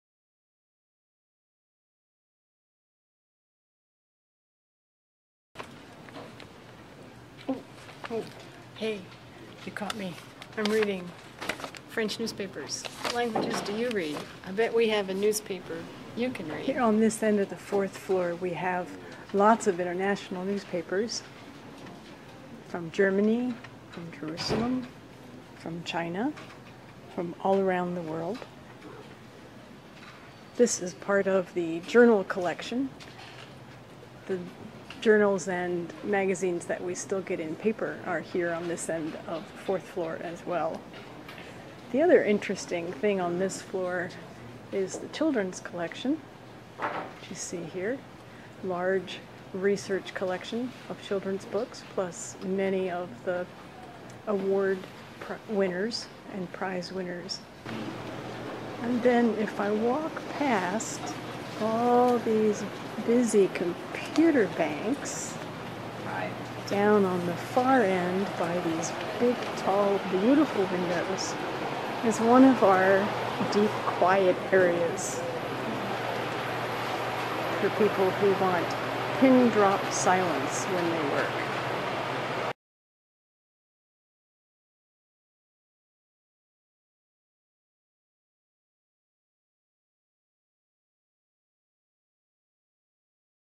4th floor 2013 final
More about the 4th floor. Part of the Video Tour of Alden Library.